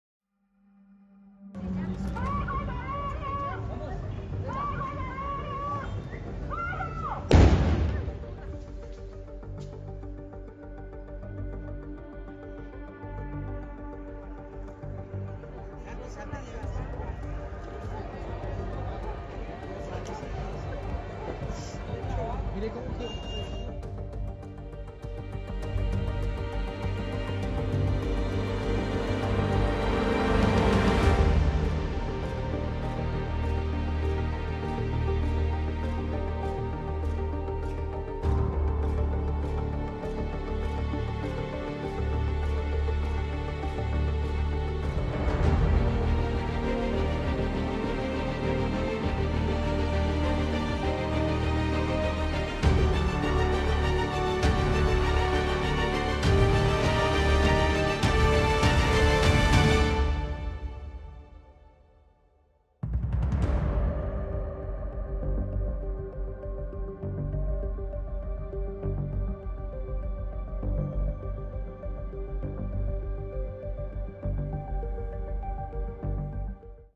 Policía encontró y controló dos granadas ubicadas en el centro de Bogotá | El Espectador
Durante la tarde de este 27 de julio, la Unidad Antiexplosivos de la Policía de Bogotá confirmó el hallazgo de dos granadas en el barrio San Bernardo, en el centro de Bogotá. Las autoridades acordonaron la zona y esperan hacer una detonación controlada para evitar comprometer la seguridad de los ciudadanos.